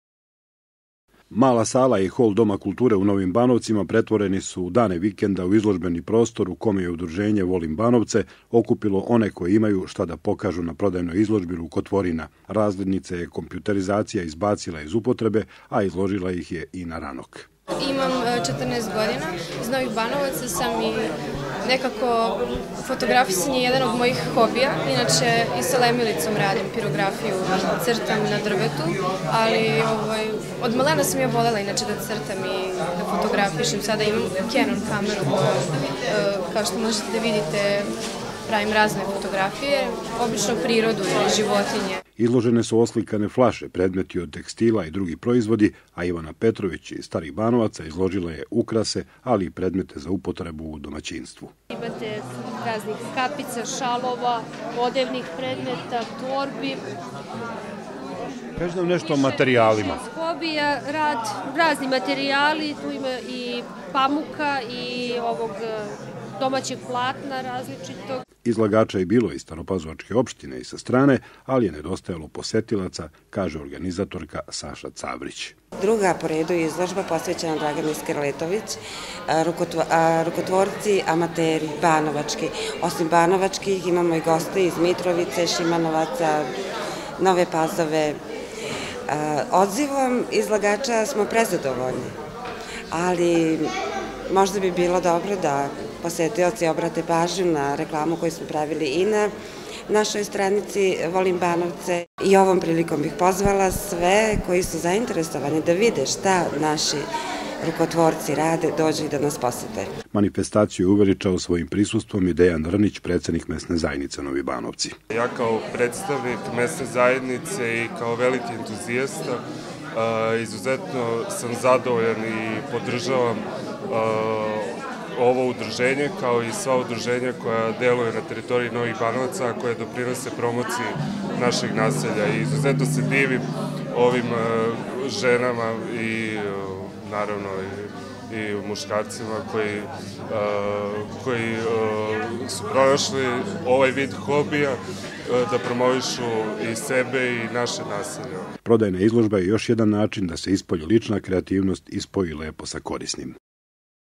Izložba rukotvorina Udruženje građana „Volim Banovce“
U cilju promovisanja kreativnosti, druženja i promocije seoskog turizma, Udruženje građana „Volim Banovce“ organizovalo u subotu i nedelju izložbu rukotvorina. Izložba je posvećena Dragani Skerletović, preminuloj članici udruženja u čiju čast je ustanovljena i nagrada.